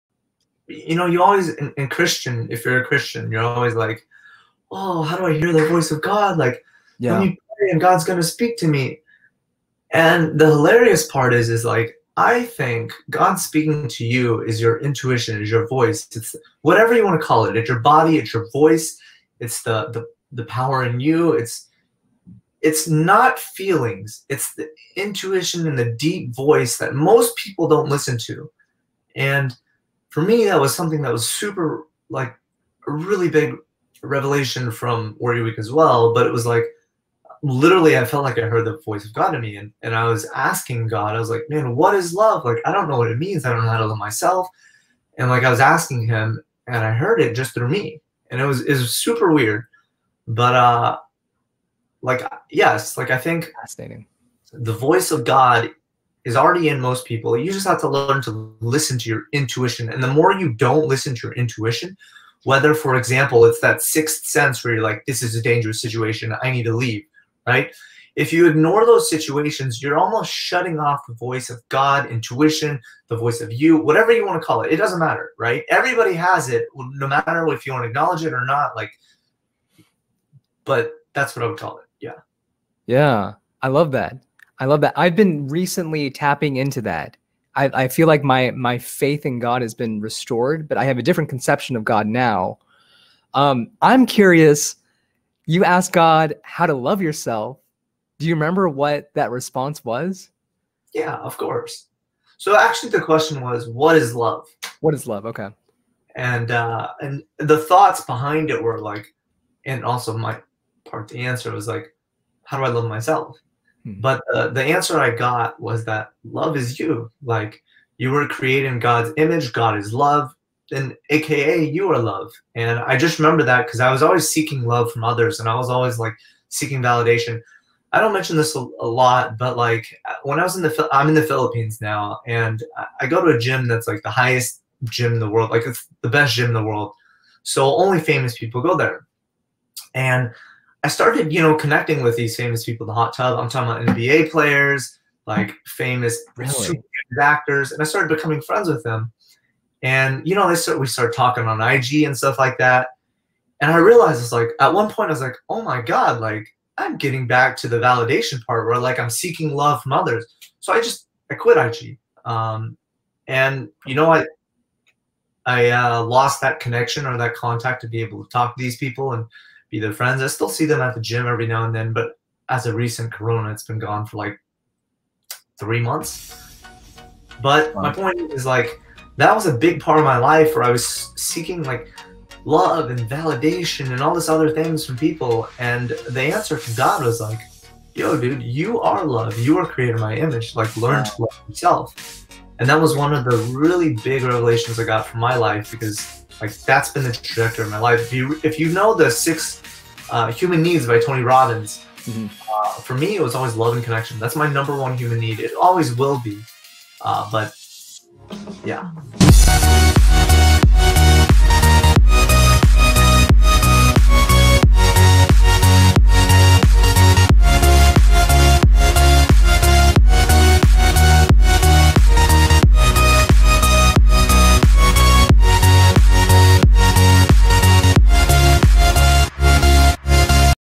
Joseph Wang on Hearing God and "You are Love"
E-commerce entrepreneur Joseph Wang talks about hearing the voice of God and receiving the revelation that you are love.